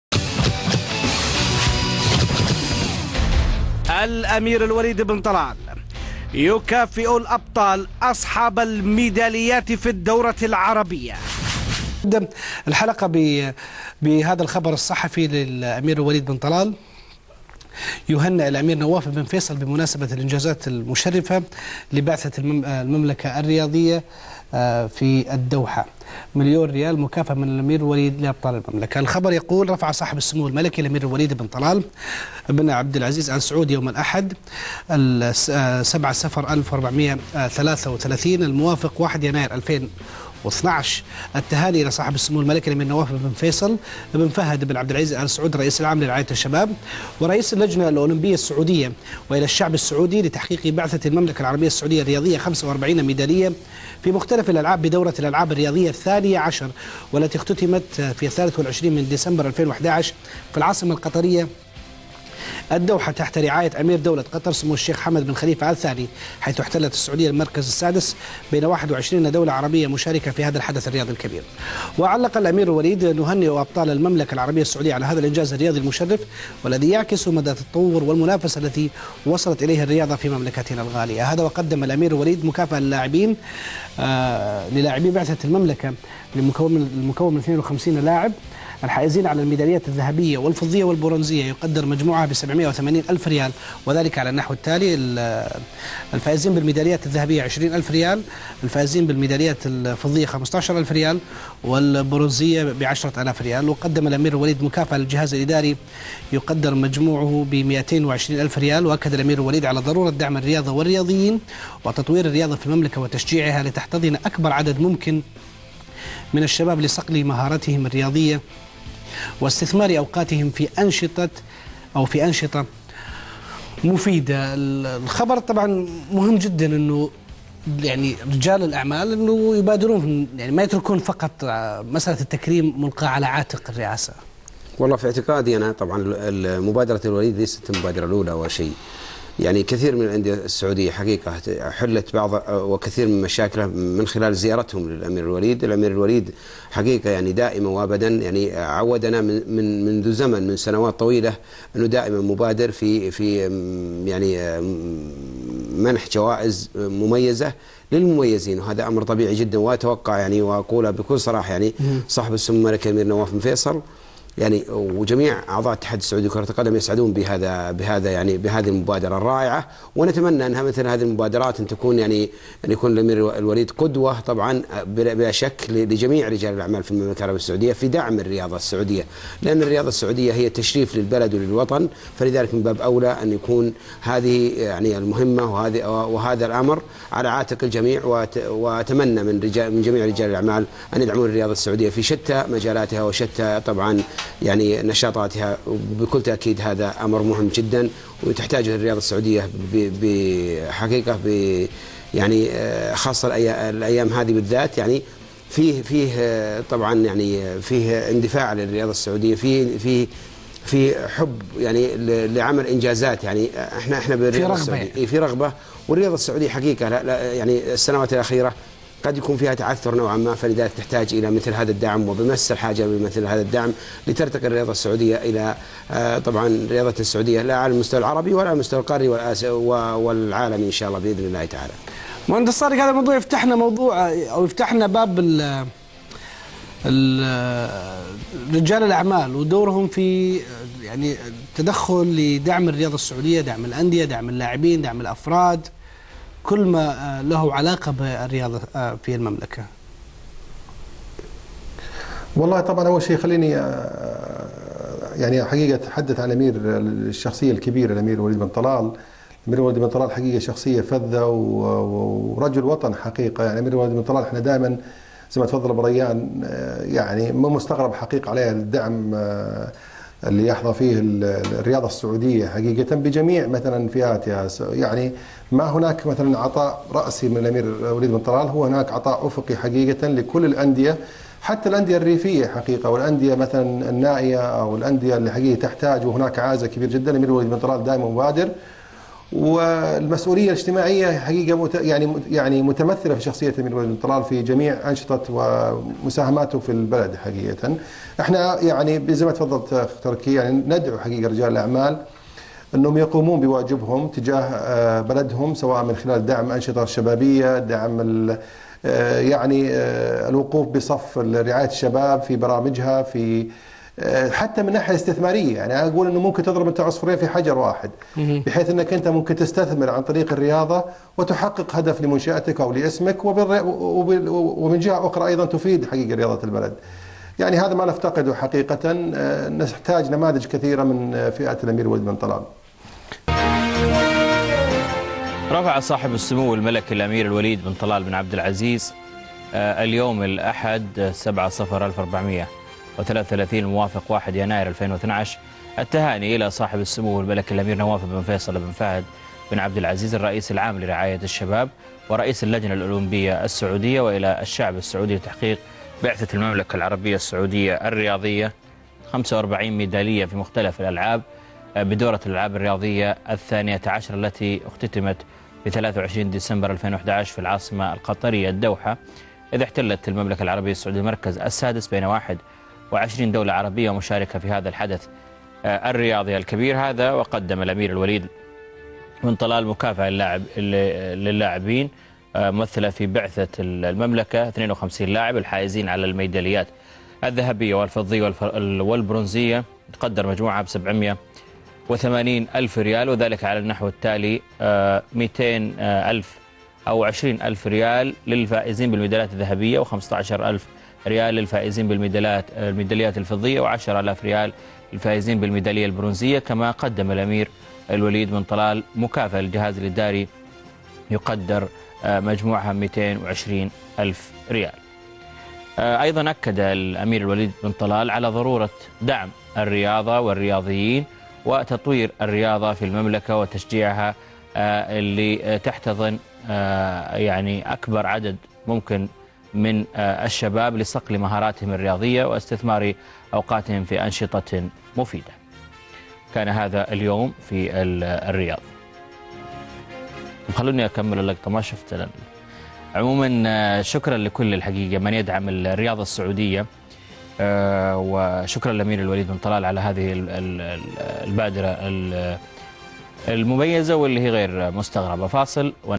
مليون ريال مكافئة من الوليد بن طلال لأبطال المملكة
الأمير الوليد بن طلال يهنى الأمير نواف بن فيصل بمناسبة الإنجازات المشرفة لبعثة المملكة الرياضية في الدوحة

مليون ريال مكافئة من الأمير الوليد لأبطال المملكة

رفع صاحب السمو الملكي الأمير الوليد بن طلال بن عبدالعزيز آل سعود، يوم الأحد 7 صفر 1433هـ المـــوافق 1 يناير 2012م، التهاني إلى صاحب السمو الملكي الأمير نواف بن فيصل بن فهد بن عبدالعزيز آل سعود الرئيس العام لرعاية الشباب ورئيس اللجنة الاولمبية السعودية، ووإلى الشعب السعودي لتحقيق بعثة المملكة العربية السعودية الرياضية 45 ميدالية في مختلف الألعاب بدورة الألعاب الرياضية الثانية عشرة التي اختتمت 23 ديسمبر 2011م في العاصمة القطرية الدوحة تحت رعاية أمير دولة قطر سمو الشيخ حمد بن خليفة آل ثاني، حيث احتلت السعودية المركز السادس بين 21 دولة عربية مشاركة في هذا الحدث الرياضي الكبير.

وعلق الأمير الوليد: "نهنئ أبطال المملكة العربية السعودية على هذا الإنجاز الرياضي المشرف الذي يعكس مدى التطور والمنافسة الذي وصلت إليه الرياضة في مملكتنا الغالية".
Prince Alwaleed Bin Talal Congratulates Prince Nawaf Bin Faisal on Saudi Team's Outstanding Performance in the 12th Pan Arab Games in Doha

SR1 Million Gift From Prince Alwaleed to Team Members

HRH Prince Alwaleed Bin Talal Bin Abdulaziz Alsaud, congratulated HRH Prince Nawaf Bin Faisal Bin Fahad Bin Abdulaziz Alsaud, General President of Youth Welfare of Saudi Arabia and Chairman of Saudi Arabian Olympic Committee for the national team's outstanding performance in the 12th Pan Arab Games which was concluded in Doha, Qatar on Friday December 23rd 2011 under the patronage of HH Sheikh Hamad Bin Khalifa Al Thani Emir of Qatar. The Saudi team came in 6th place in a fierce competition with 21 Arab countries with a total of 45 medals in different competitions.

Prince Alwaleed commented: "We congratulate our champions for this great achievement that reflects the advancement made in sport in the Kingdom of Saudi Arabia."